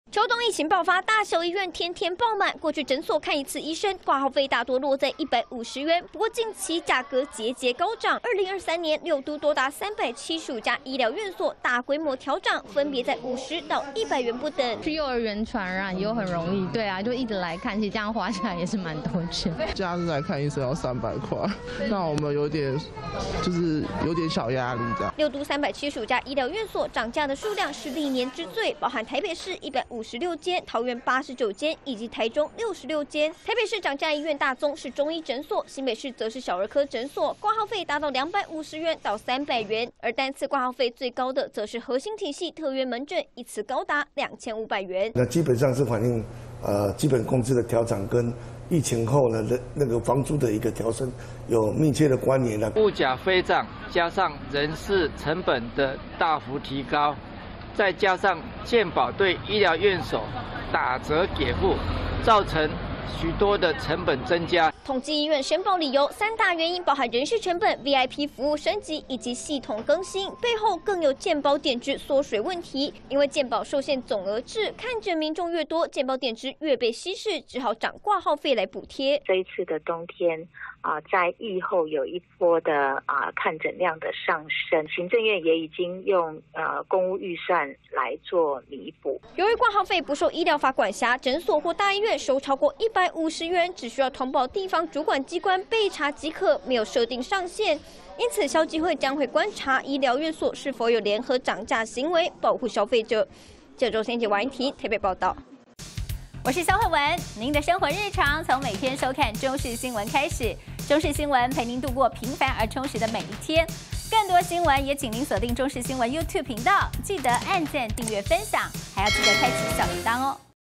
醫療院所"掛號費狂漲" 六都"375間"變貴.最高2500元!│中視新聞 20231218
過去看診，掛號費多落在150元，不過這個價格卻"漸漸消失"，2023年六都 漲價醫療院所，共計375間，達到歷年之最，現在到診所看個病, 光掛號費可能就是250元到300元!分析背後原因，竟是因為看診人潮多，"健保點值"被稀釋，診所只好漲掛號費來做補貼。